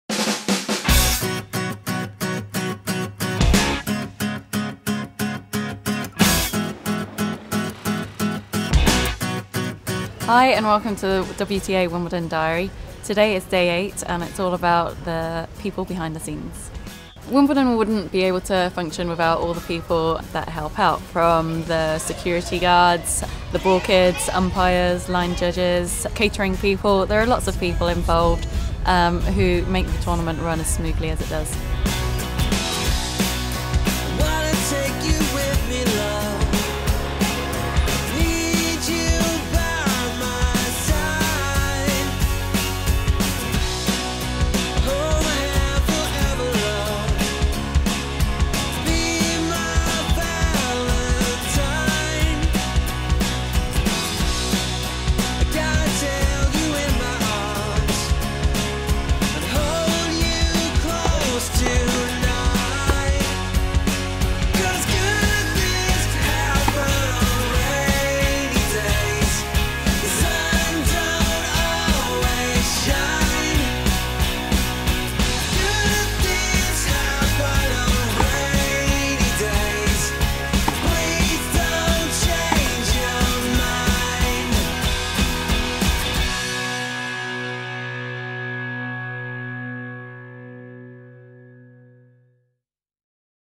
The WTA Wimbledon Diaries: Day Eight
It takes a lot of people to make Wimbledon run as smoothly as it does. From umpires, ball kids, security, catering staff and more, the grounds staff & employees behind the scenes help to make this one of the most enjoyable sporting events in the world.